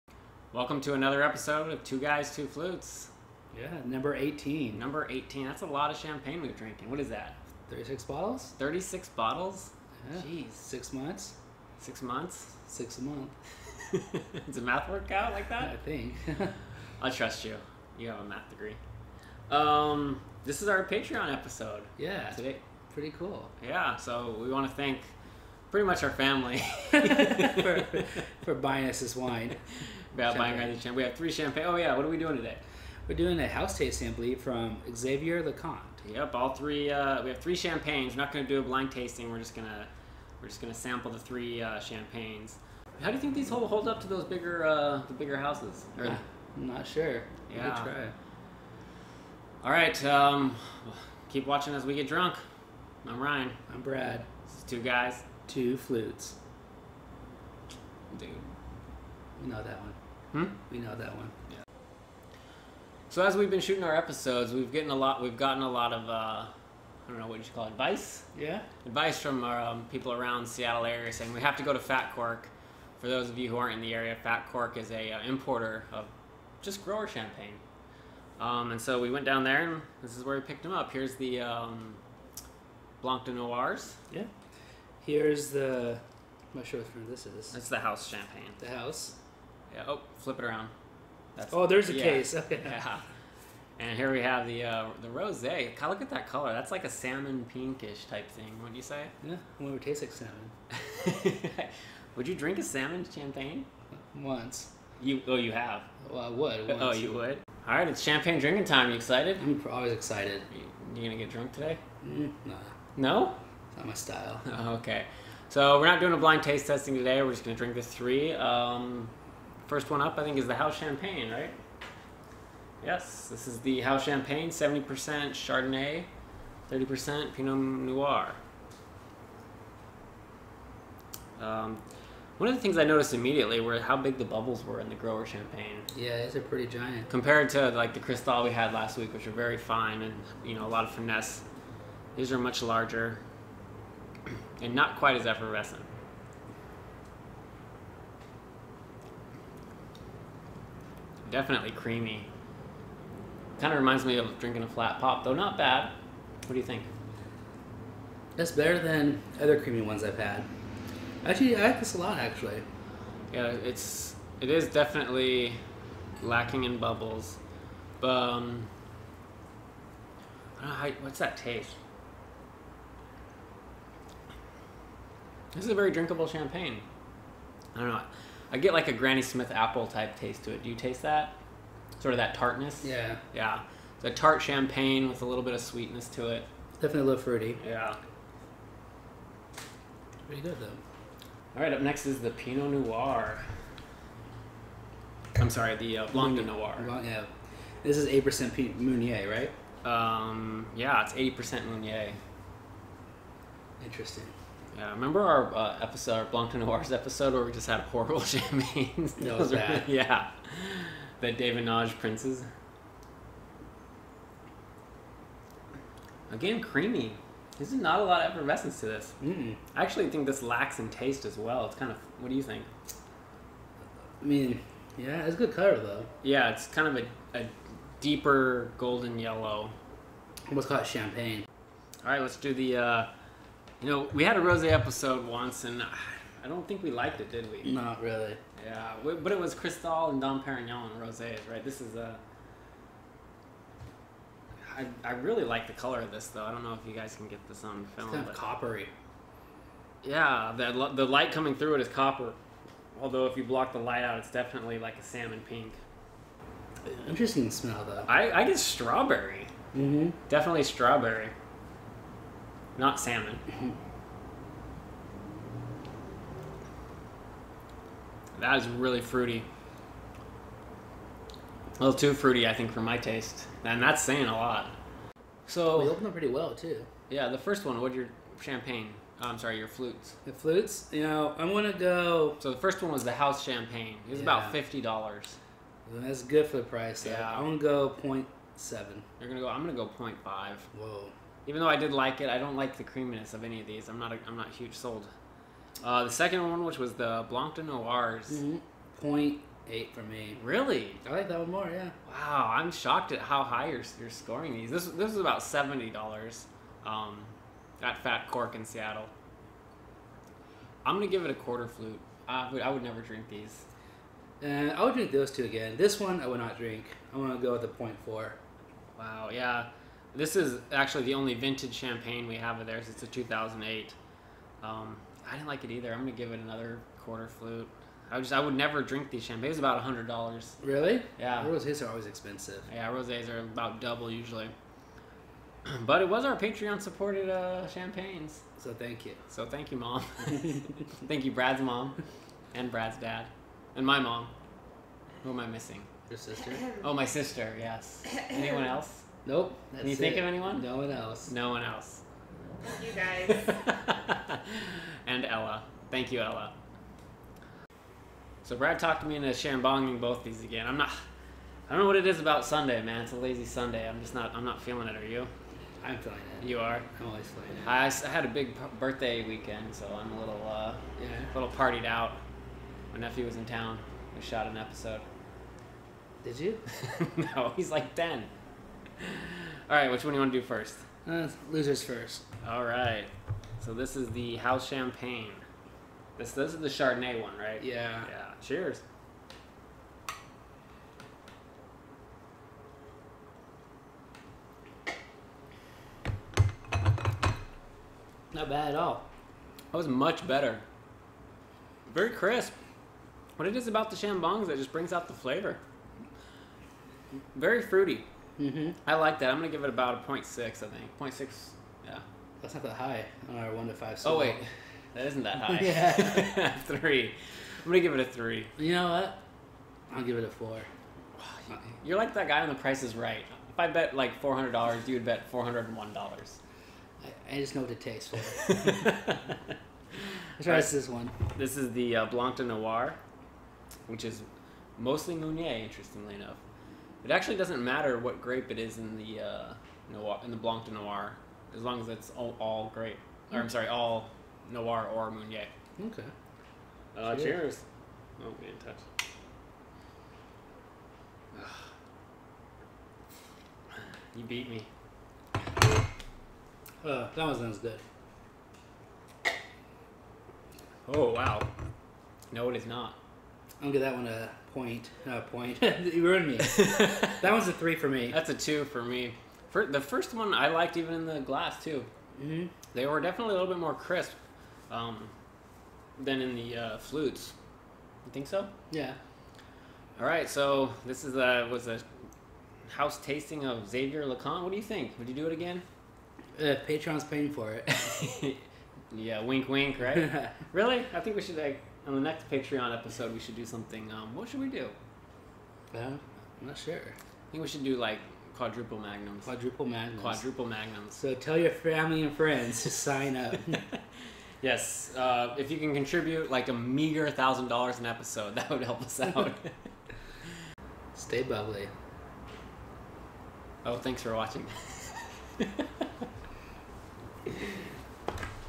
Episode 18: Xavier Leconte
A grower champagne tasting from Xavier Leconte.  for exclusive champagnes.